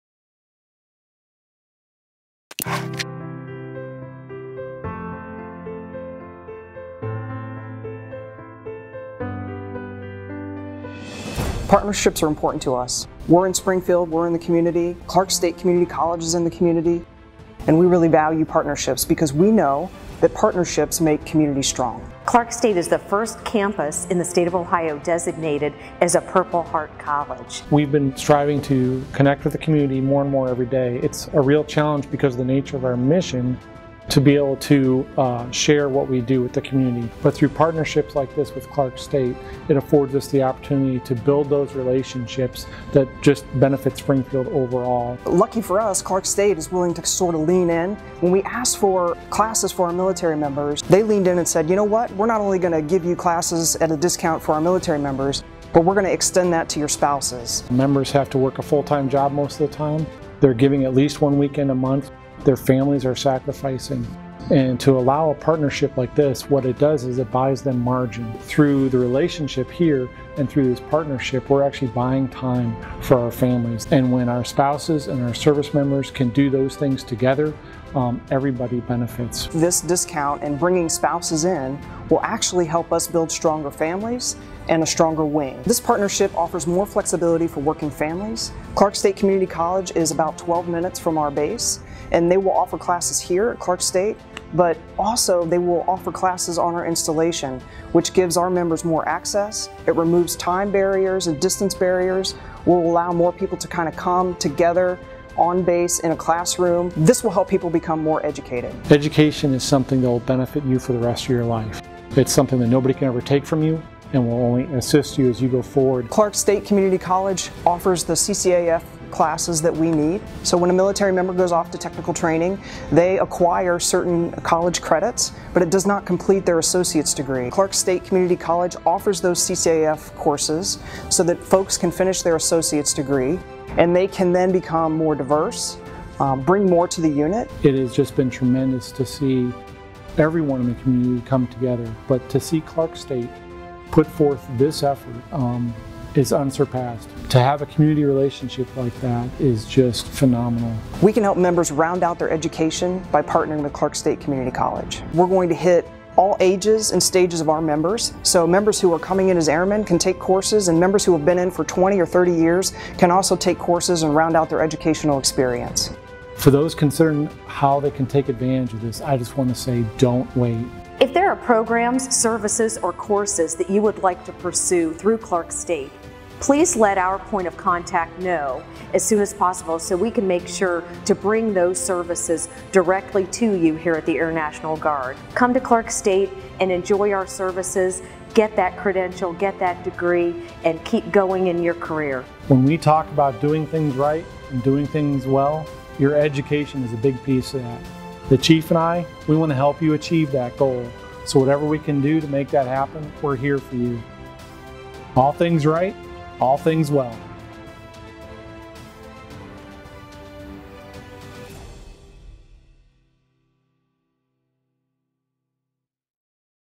Clark State Community College's Partnership with the Air National Guard
Clark State Community College partnered with the 178th Wing of the Air National Guard to better serve our military and veterans. Along with educational opportunities are potential discounts for military and their spouses.